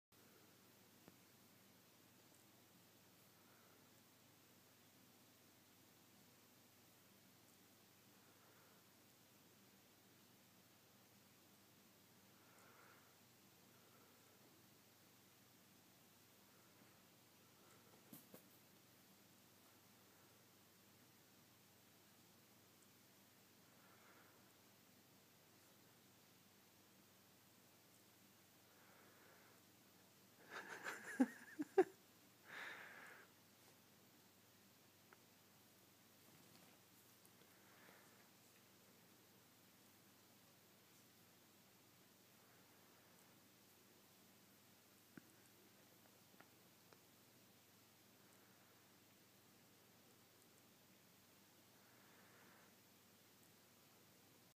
Siamese in a mirror
Sammy and Bruno discover a mirror